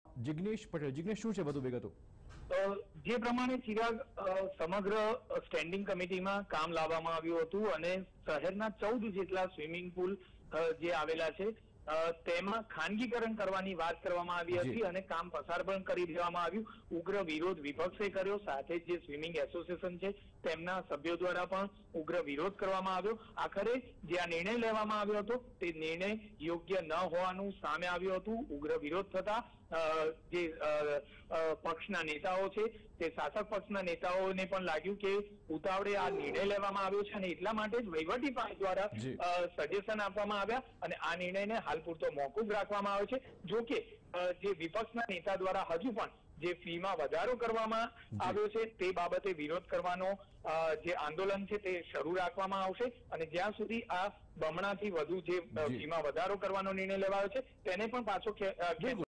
AMC run swimming pools will not be privatised, Ahmedabad - Tv9
AMC run swimming pools will not be privatised, Ahmedabad